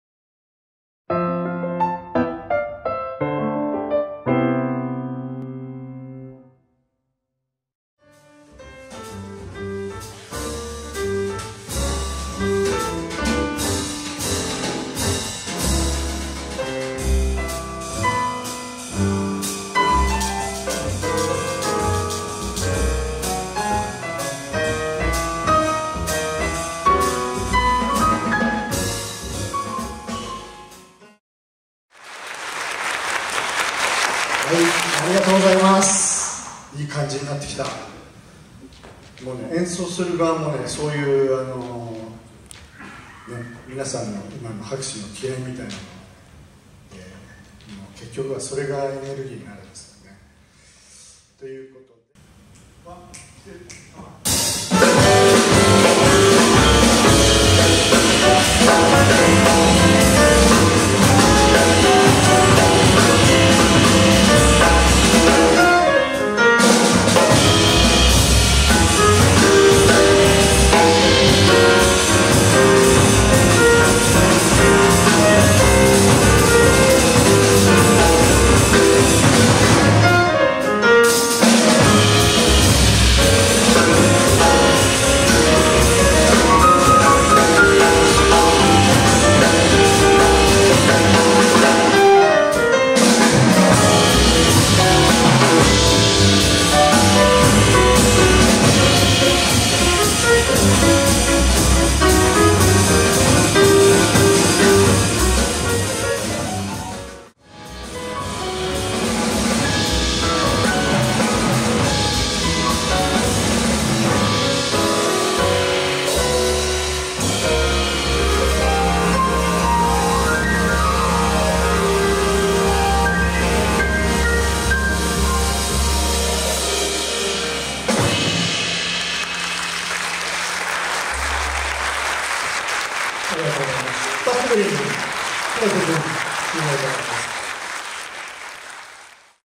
プラザ・アフタヌーンジャズVol.1（椎名豊トリオ）
開催：2021年5月30日（日）
出演：椎名 豊 トリオ/椎名 豊（ピアノ）、パット・グリン（ベース）、広瀬 潤次（ドラム）
主催：川崎市民プラザ

2021年からシリーズ開催しているプラザ・アフタヌーンジャズ。
Vol.1では、2021年5月8日の事前講座に引き続き、椎名豊トリオが登場し、オリジナル曲とスタンダードナンバーをたっぷり聴かせてくれました。

MCではピアノの椎名豊氏がジャズの楽しみ方として、手拍子の入れ方やスイングのリズムをレクチャー。またオリジナル曲のタイトルに込めた思いや作曲秘話などを語りました。

コンサートの様子をダイジェストでお楽しみください。

映像制作：川崎市民プラザ